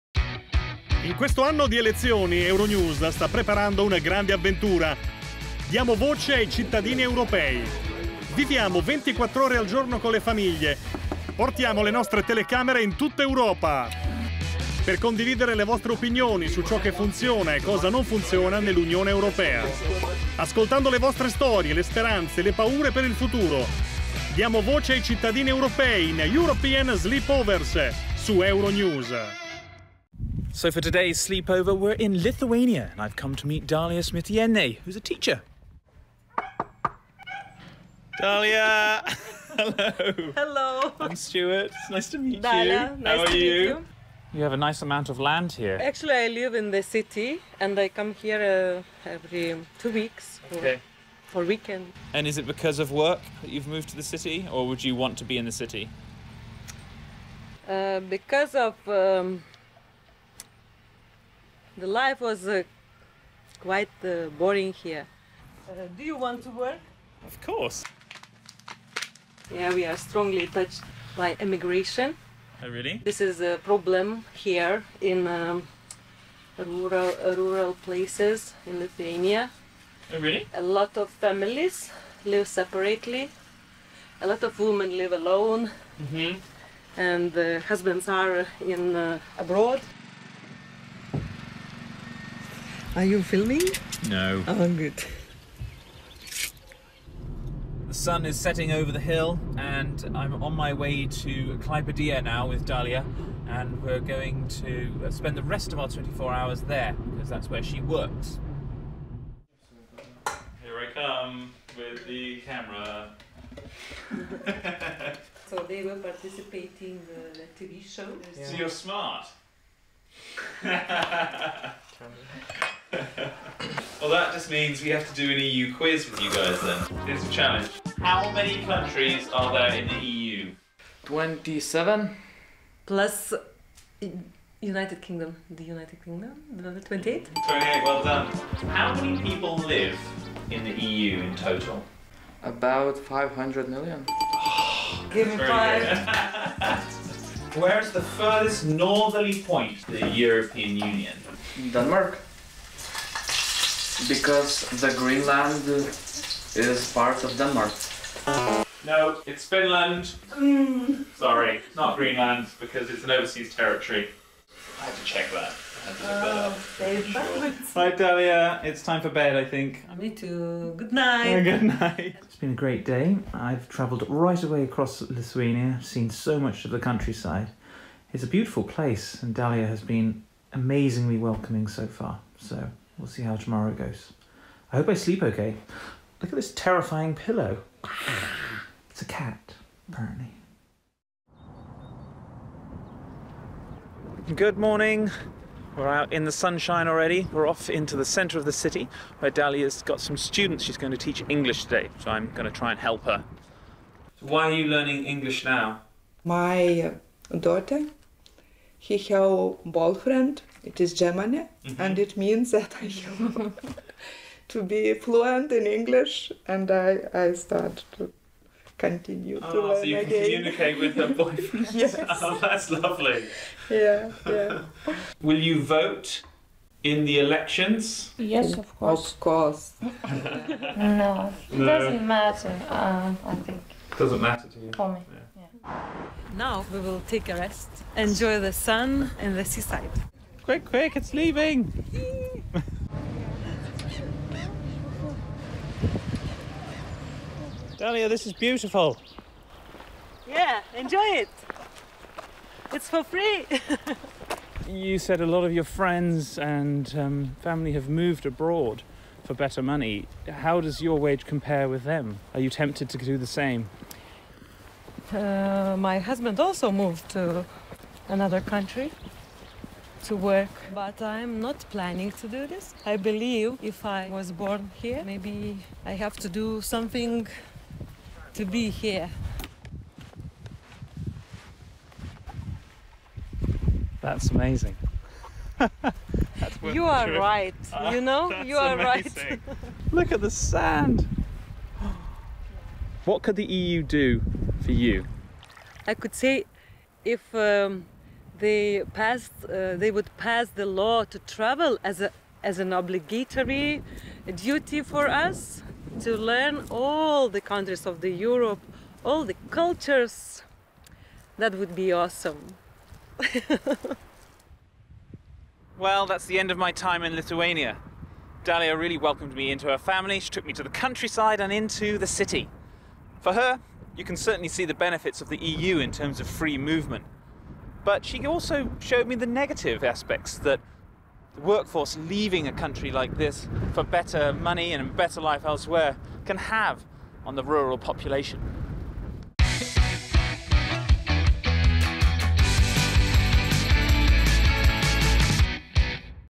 European Sleepover - Lituania ed Europa: tra speranze e dubbi
Viaggio di "Sleepover" in Lituania alla vigilia delle europee. Tra speranze e dubbi…